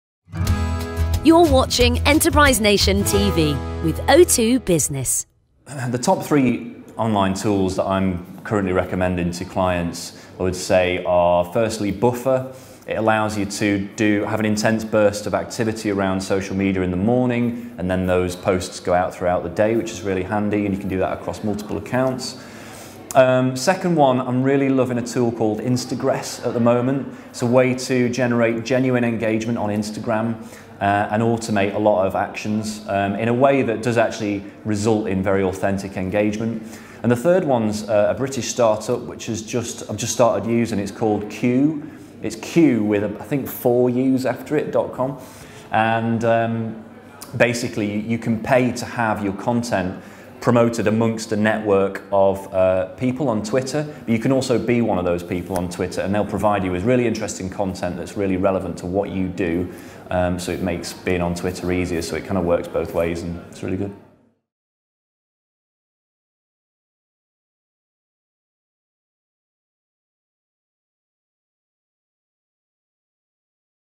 Three great social media tools for small businesses
Will Francis, founder of Harkable, shares his three favourite small business online tools.